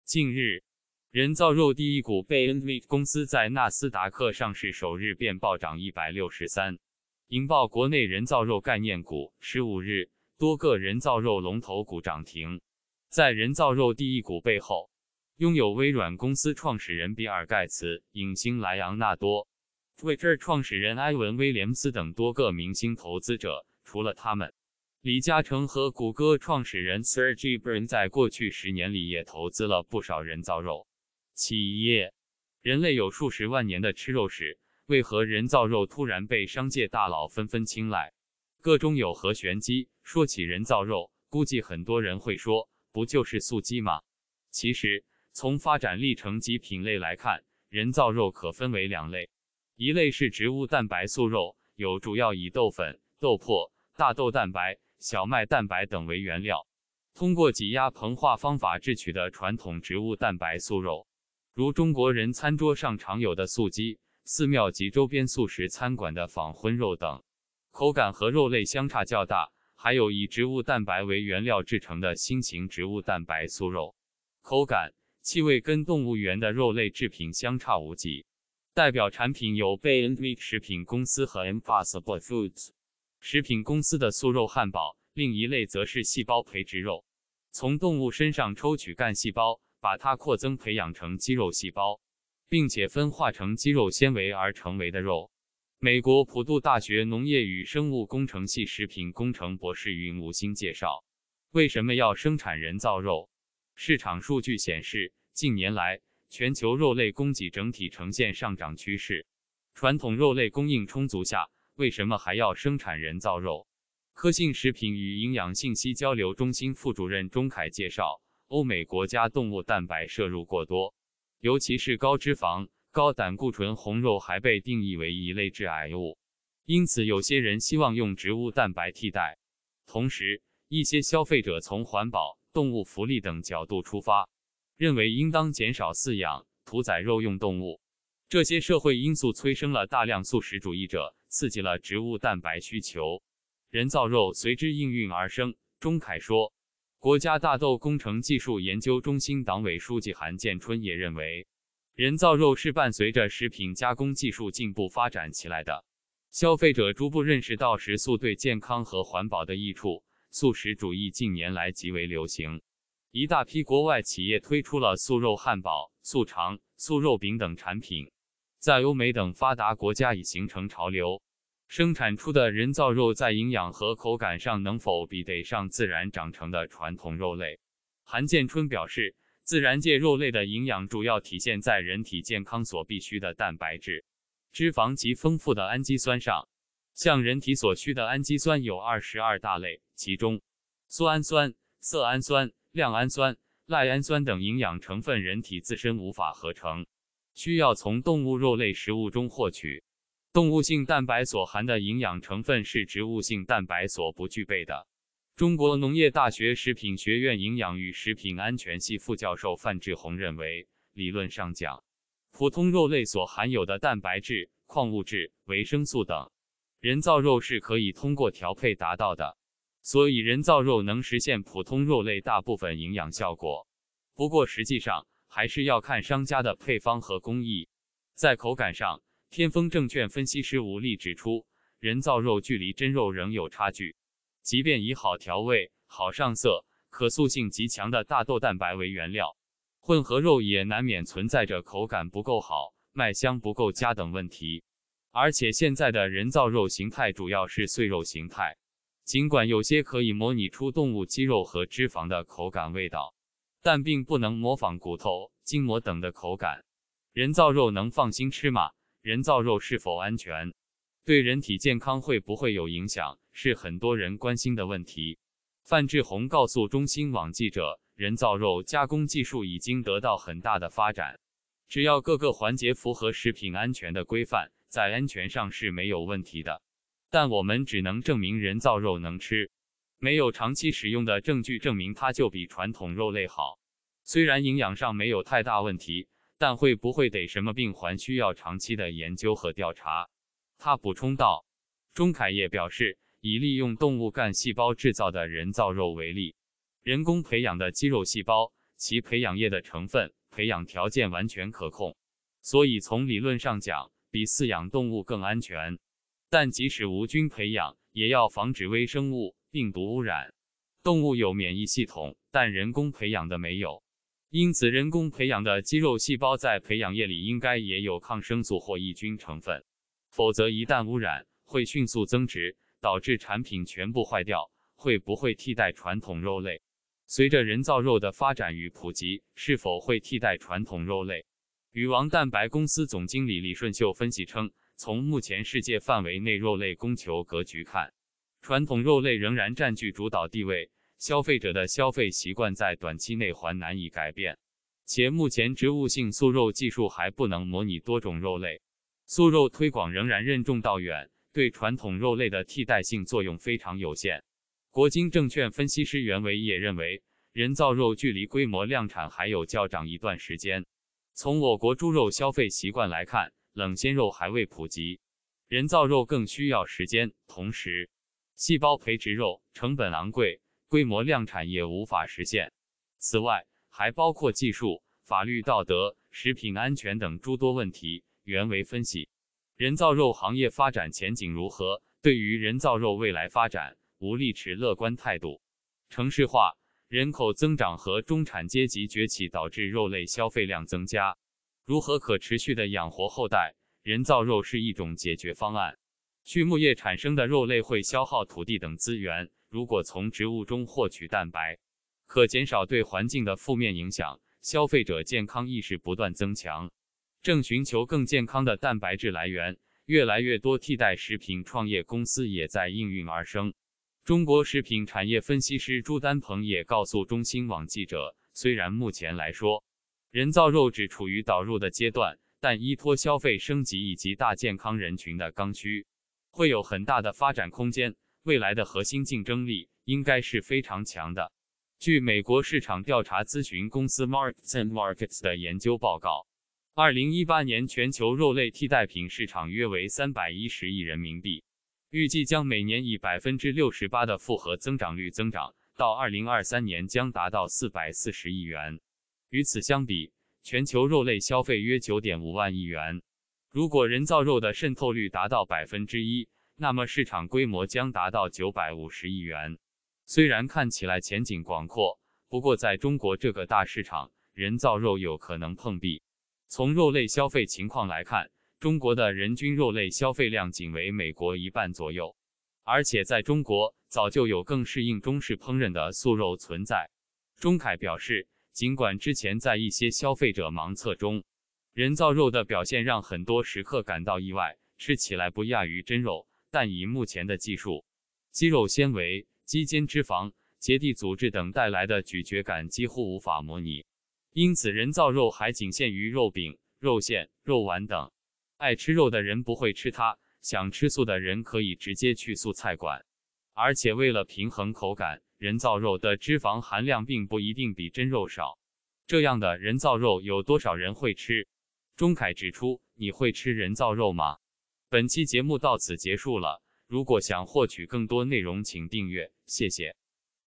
被热炒的“人造肉” 到底能不能吃？健不健康？
近日，“人造肉第一股”beyond meat公司在纳斯达克上市首日便暴涨163%，引爆国内“人造肉”概念股，15日，多个“人造肉”龙头股涨停。
在“人造肉第一股”背后，拥有微软公司创始人比尔盖茨、影星……